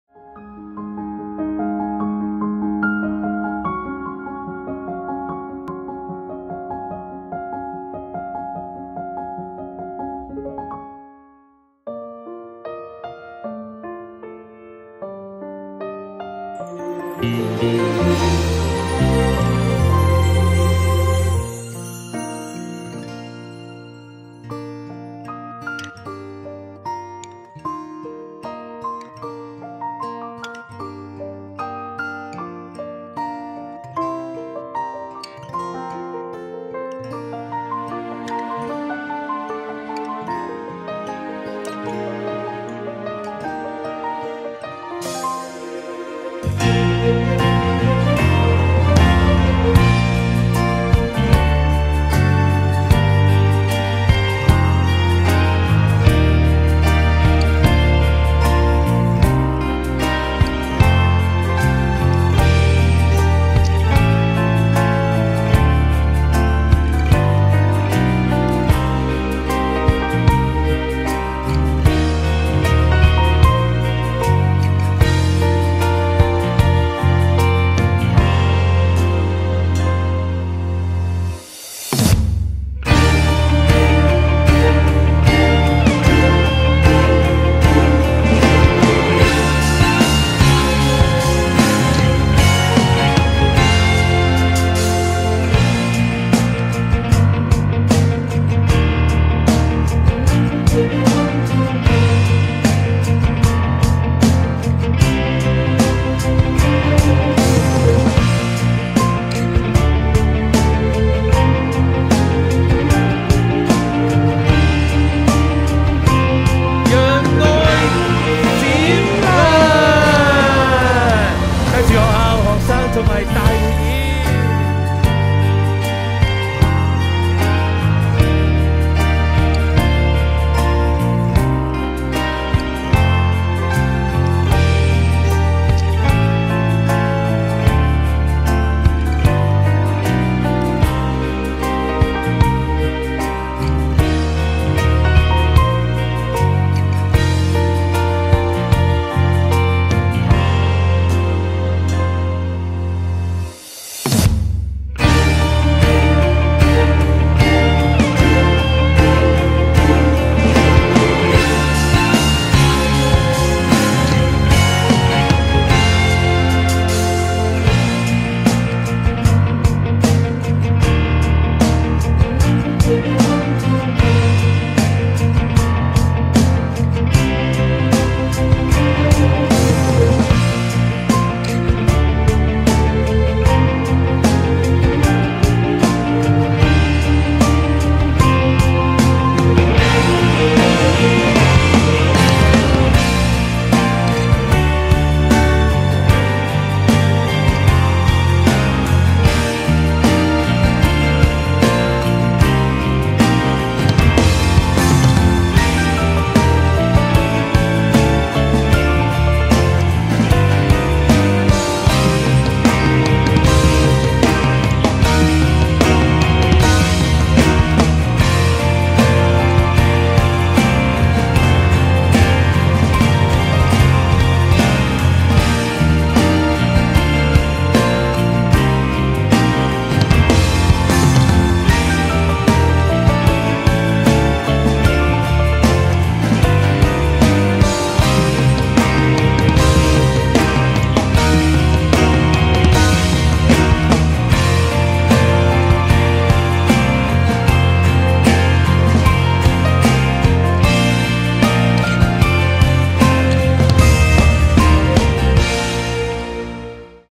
特殊學校學生綜藝大匯演 - 讓愛閃亮 2022精華片段
特殊學校學生綜藝大匯演 - 讓愛閃亮 2022於八月十一日上午在灣仔伊利沙伯體育館舉行。表演項目包括樂器演奏、舞龍、武術及輪椅舞等等。為讓公眾，包括聽障及視障人士能夠欣賞特殊學校學生的精彩演出，大會透過專屬網站作無障礙(手語翻譯及口述影像)進行直播。